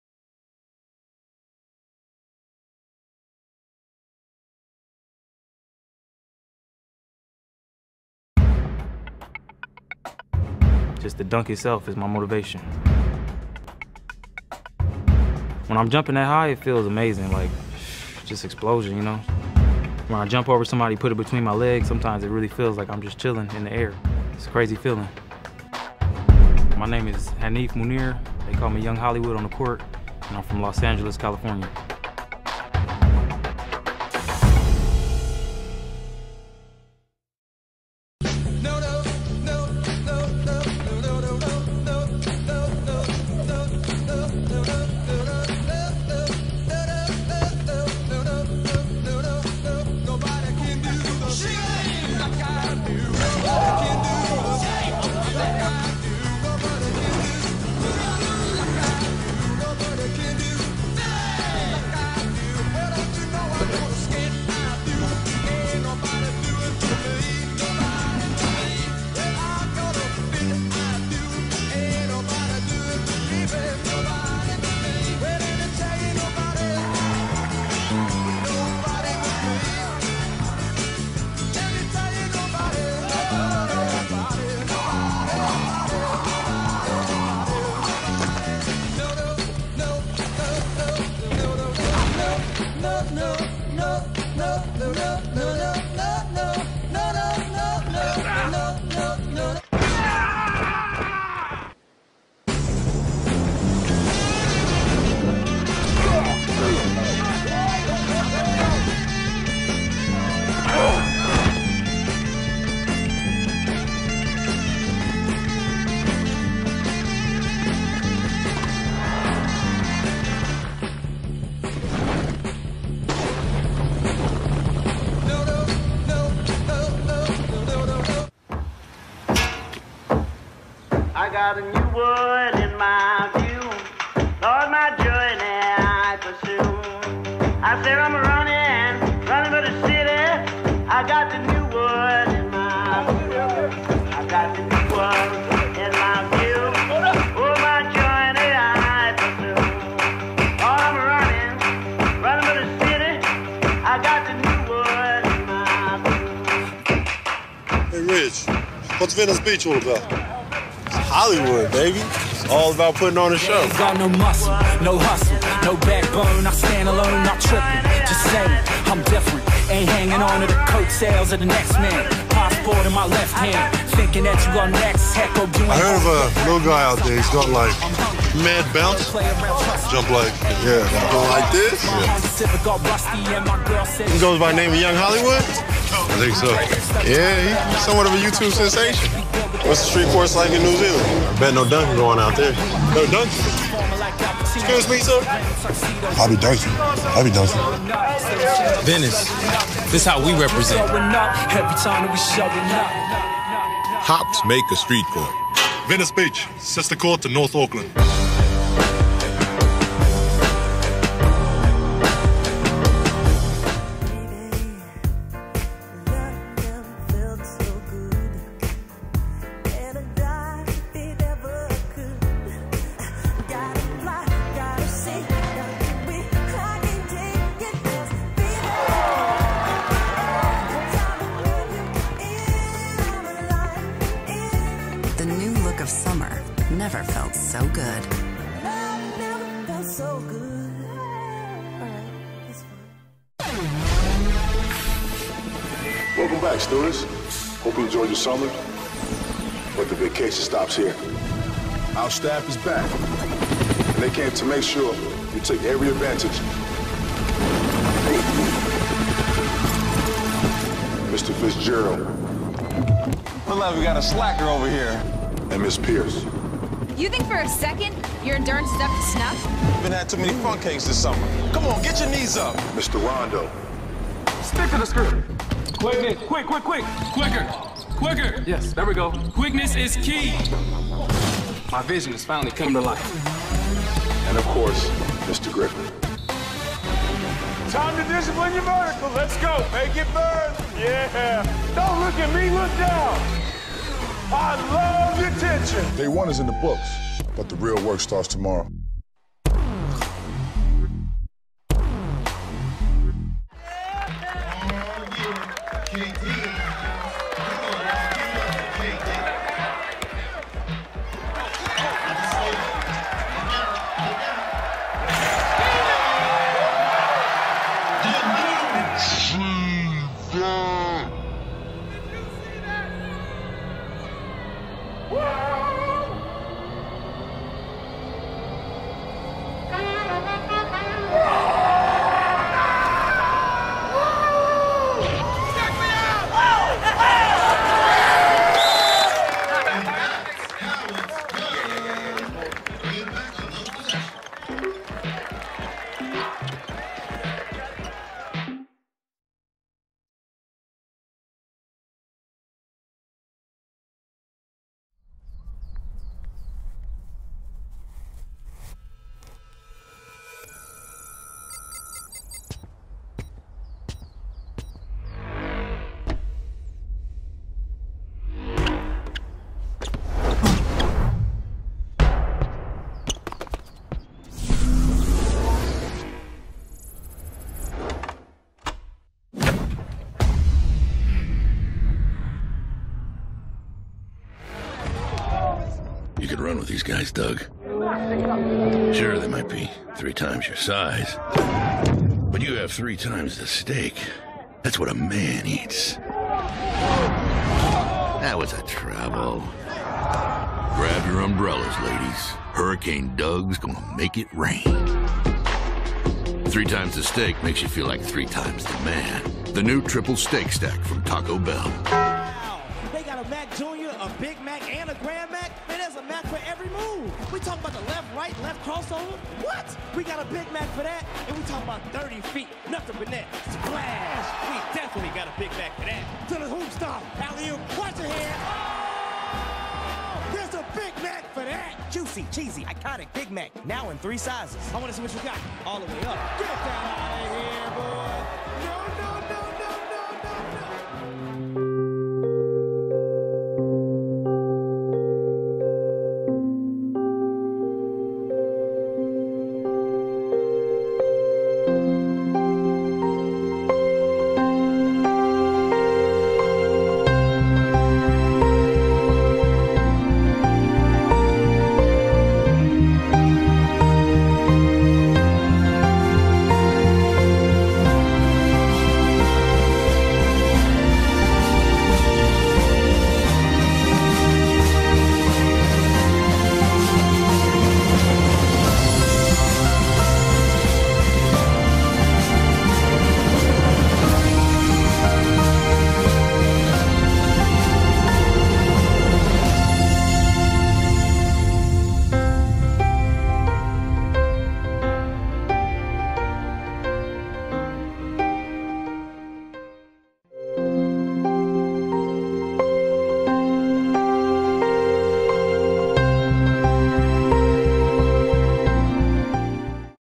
Haneef Munir - Commercial Reel
A short resume of commercial’s I’ve been in.